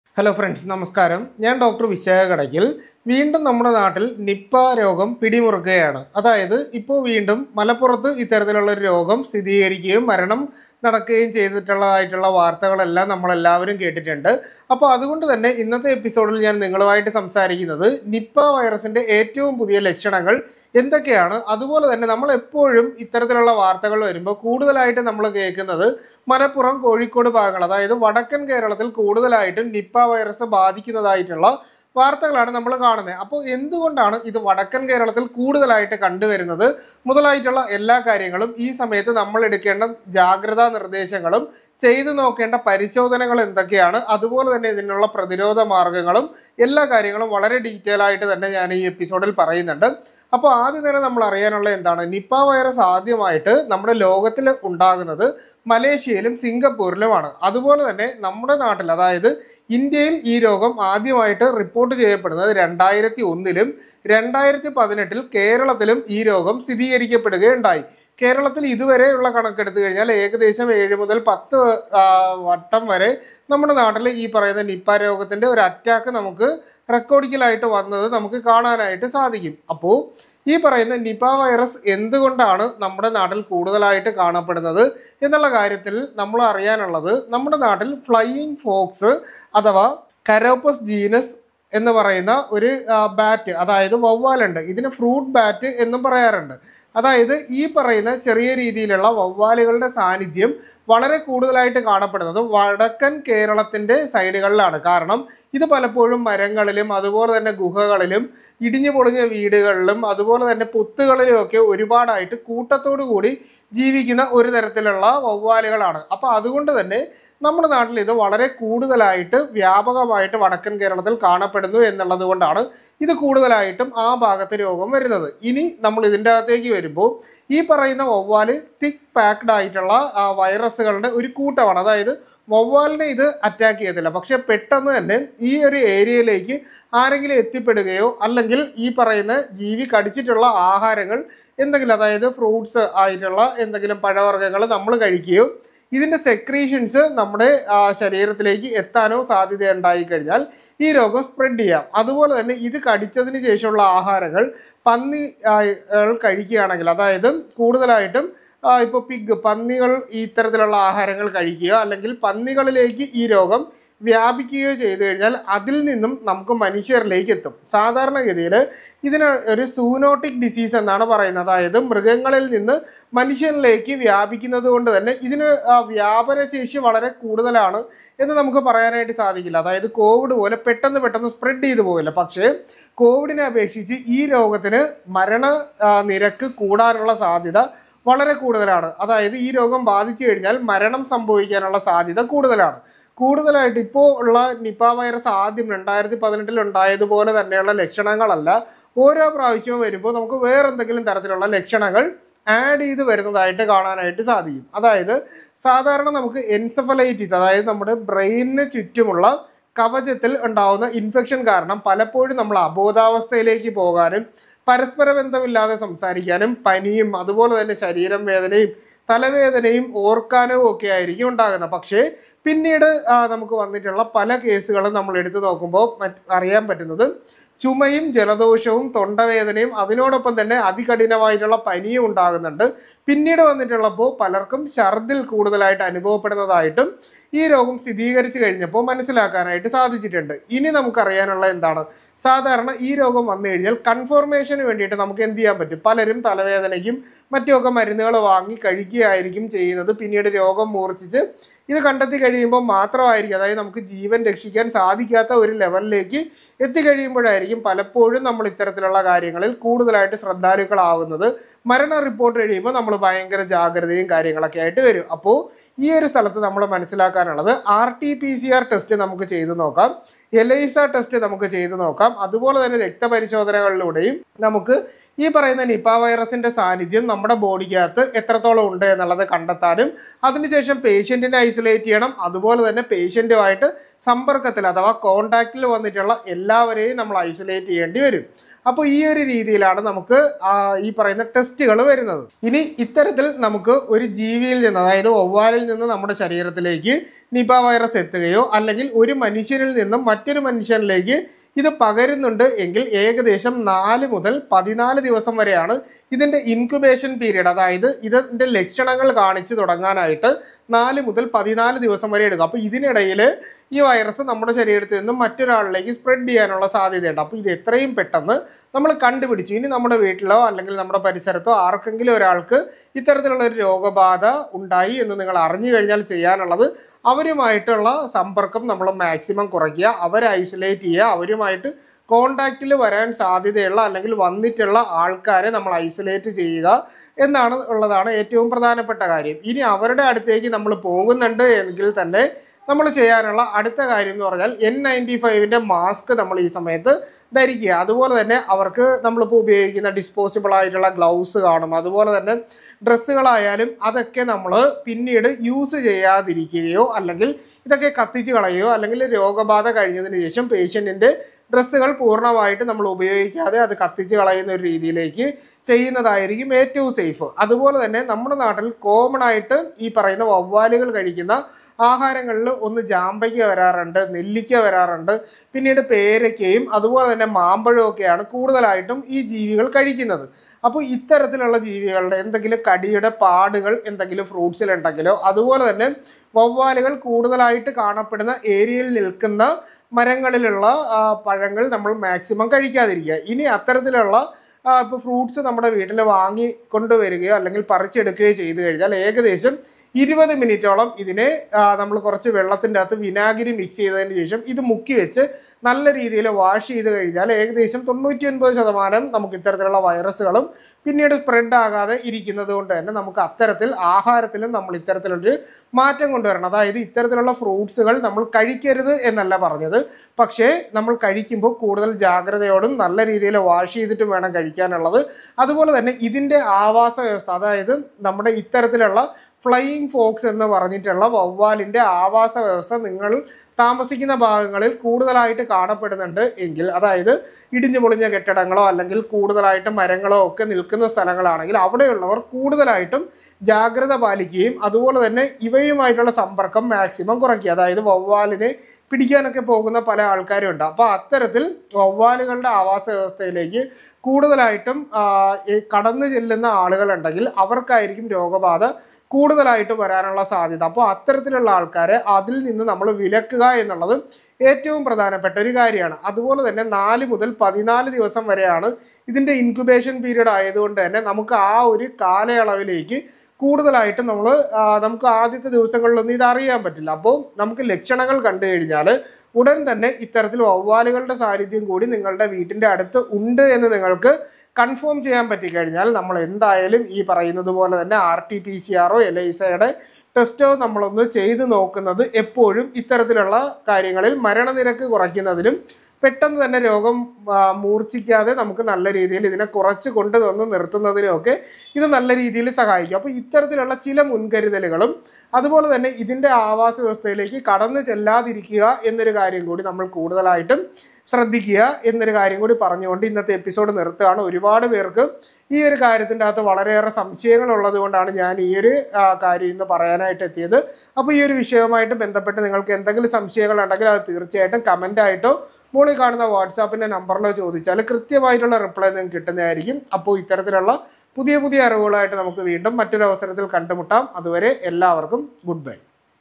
നിപ ഏറ്റവും പുതിയ രോഗ ലക്ഷണങ്ങൾ | എന്ത് കൊണ്ട് വടക്കൻ കേരളത്തിൽ കൂടുതലായി ഈ രോഗം വരുന്നു.?
Dr.VISAKH KADAKKAL
BAMS,MS (Ayu)
Chief Medical Consultant
Sri Padmanabha Ayurveda Speciality Hospital, Altharamoodu, Kadakkal

Appointments : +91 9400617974 (Call or WhatsApp)